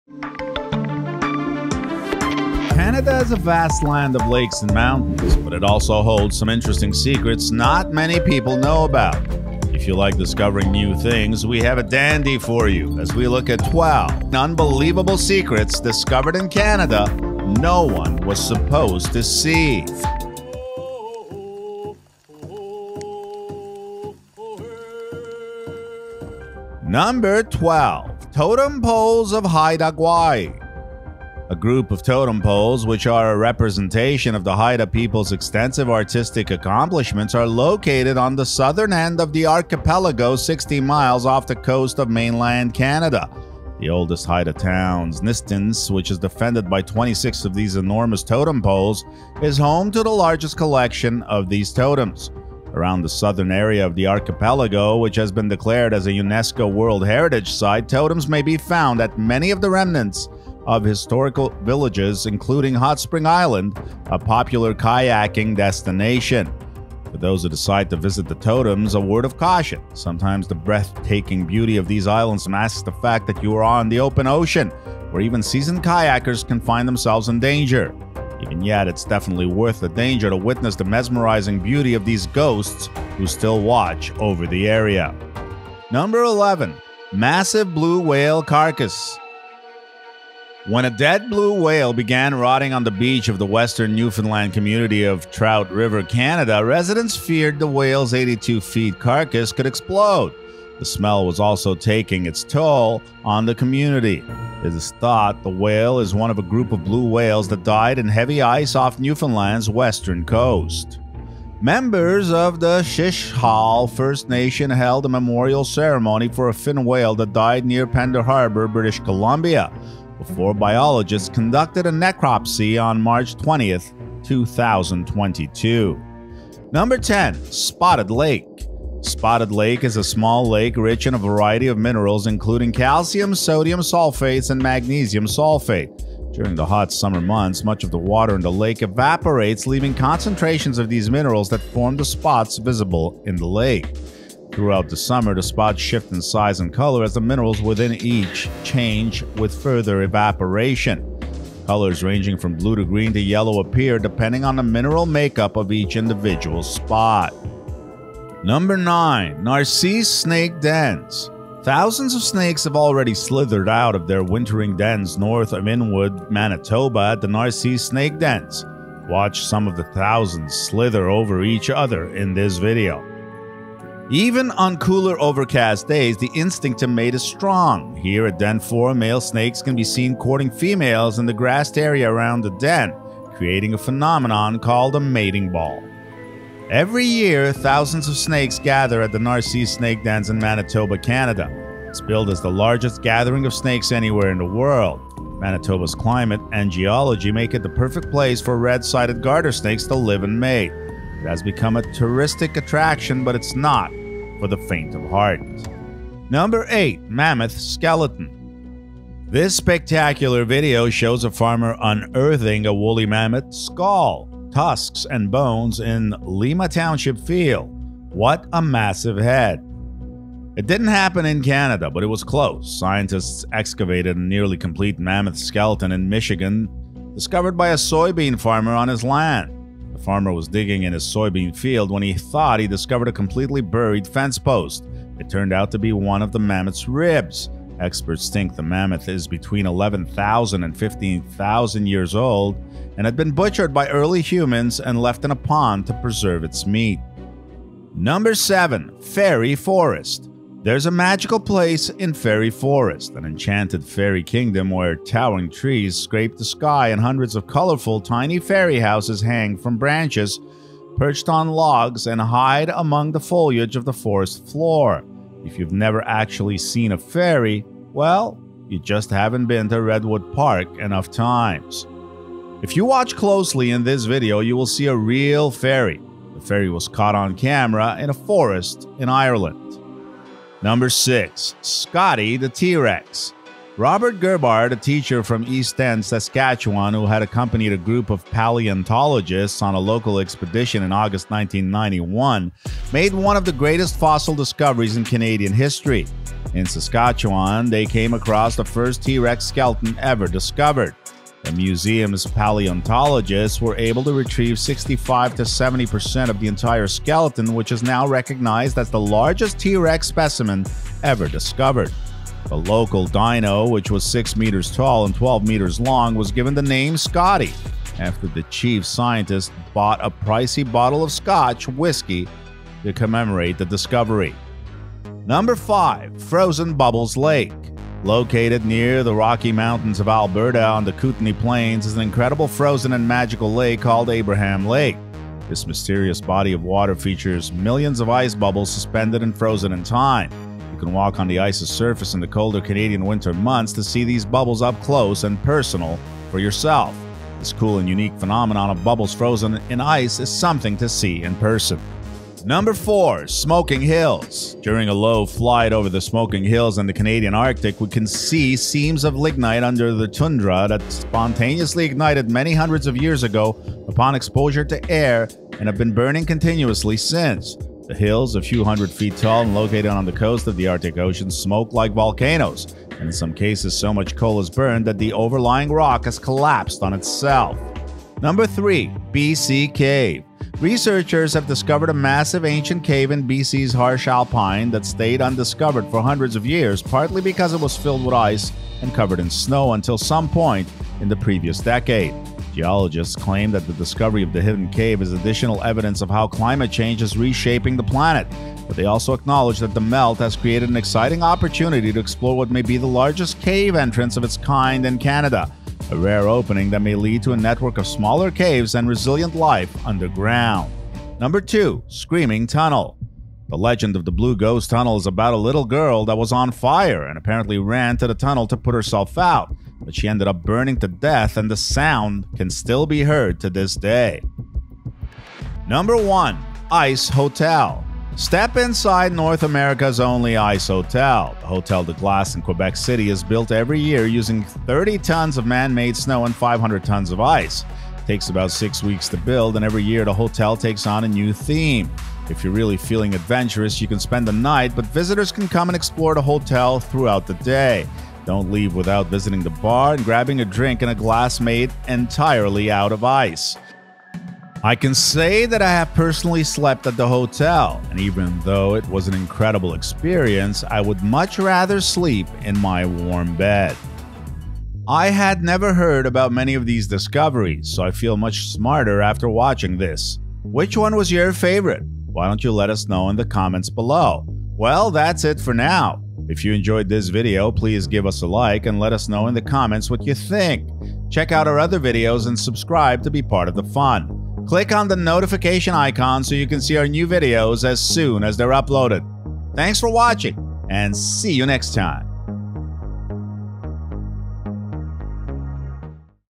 12 Unbelievable Secrets Discovered In Canada No One Was Supposed To See
Canada is a vast land of lakes and mountains, but it also holds some interesting secrets not many people know about. If you like discovering new things, we have a dandy for you, as we look at 12 unbelievable secrets discovered in Canada, no one was supposed to see.

Totem Poles Of Haida Gwaii

A group of totem poles, which are a representation of the Haida people's extensive artistic accomplishments, are located on the southern end of an archipelago sixty miles off the coast of mainland Canada. The oldest Haida town Ninstints, which is defended by 26 of these enormous totem poles, is home to the largest collection of these totems. Around the Southern area of the archipelago, which has been declared as a UNESCO World Heritage Site, totems may be found at many of the remnants of historic villages, including Hotspring Island, a popular kayaking destination. For those who decide to visit the totems, a word of caution: sometimes the breathtaking beauty of these islands masks the fact that you are on the open ocean, where even seasoned kayakers can find themselves in danger.

Massive Blue Whale Carcass

When a dead blue whale began rotting on the beach of the western Newfoundland community of Trout River, Canada, residents feared the whale's 82 feet carcass could explode. The smell was also taking its toll on the community. It is thought the whale is one of a group of blue whales that died in heavy ice off Newfoundland's Western coast. Members of the Shishalh First Nation held a memorial ceremony for a fin whale that died near Pender Harbour, B.C., before biologists conducted a necropsy on March 20, 2022.